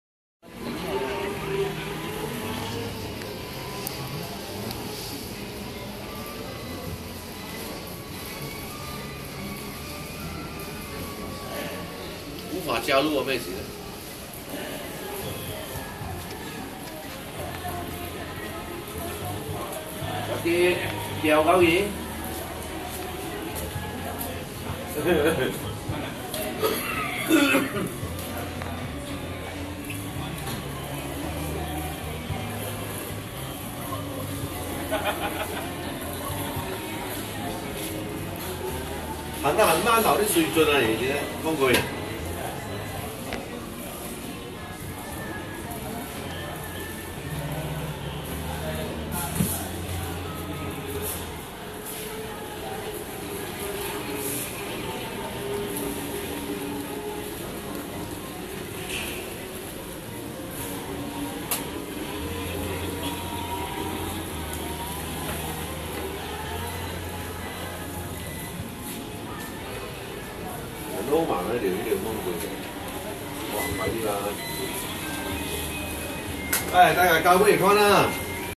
My Robin-M1 Beginner Joy, The STORE in Pandan Indah, BM Time Watch Shop
Old watch shop with very experienced repairman.
Repair all kind of watches that are repairable.

Prices very reasonable mainly due to location in a middle income area plus low running cost.

This shopping Mall is undergoing full refurbishment so all products are on sale at maximum discount,